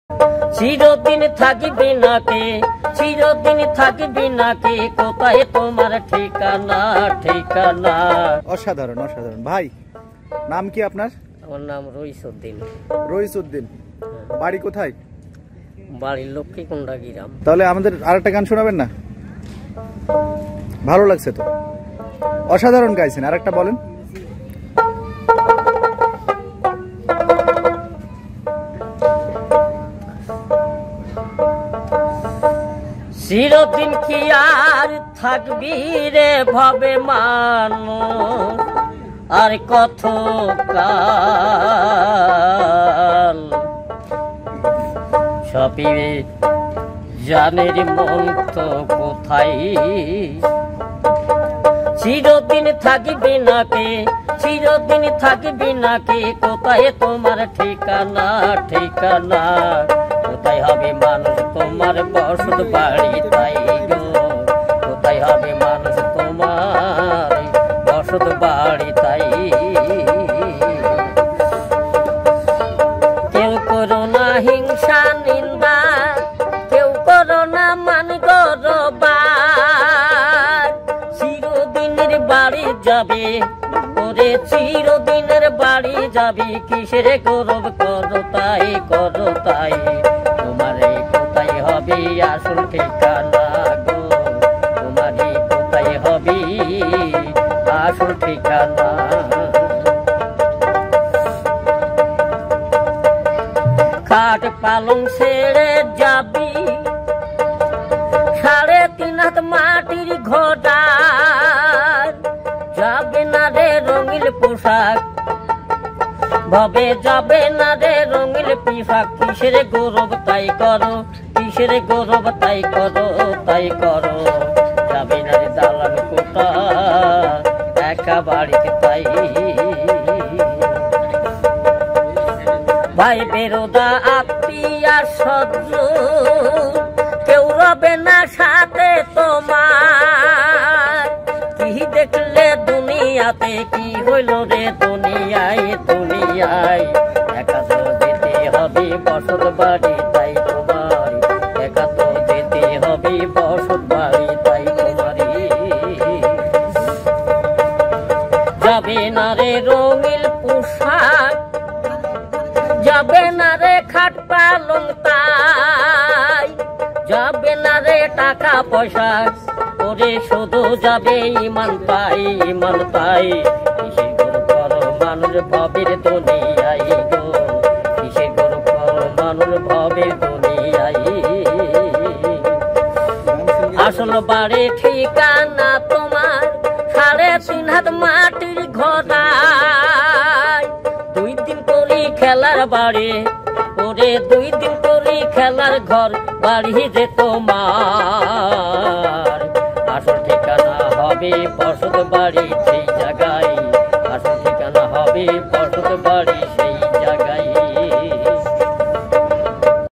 আধ্যাত্মিক যুগের গান,শুনলে হৃদয় চক্ষু খুলে যাবে।। Bangla Adunik Gan | Best Bangla Adhunik Song
আধ্যাত্মিক যুগের গান,শুনলে হৃদয় চক্ষু খুলে যাবে।। মরমী গান
শিল্পী রইজ উদ্দিন, ঈশ্বরদী, পাবনা, যার গানগুলো শুনলে হৃদয় জুড়িয়ে যায় এবং অন্তর চক্ষু খুলে যায়। তিনি ঈশ্বরদী থানার রূপপুর এবং পাকশি হার্ডিং ব্রিজ অঞ্চলে বিভিন্ন সময়ে গান গেয়ে অর্থ উপার্জন করে জীবিকা নির্বাহ করে থাকেন।